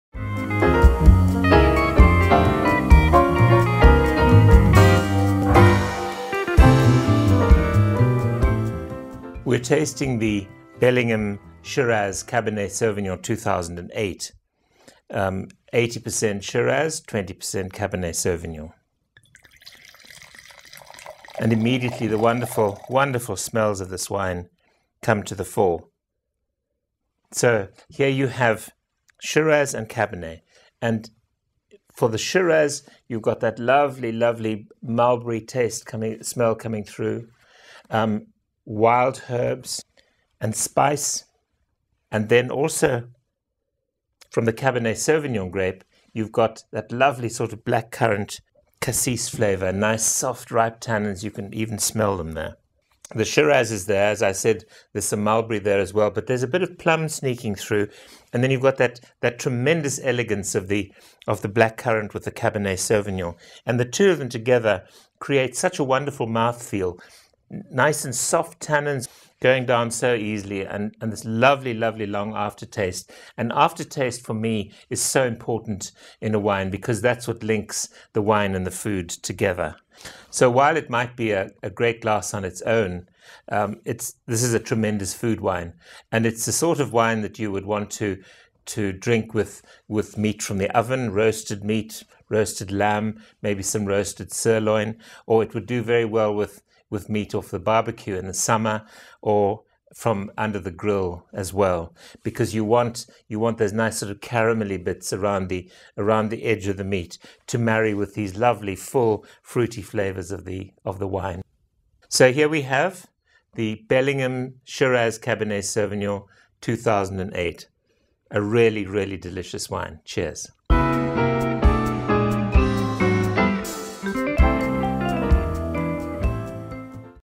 Michael's Minute: Bellingham Shiraz and Cabernet Sauvignon - 2008
In this film, Michael tastes the 2008 Shiraz (with a splash of Cabernet Sauvignon) from Bellingham.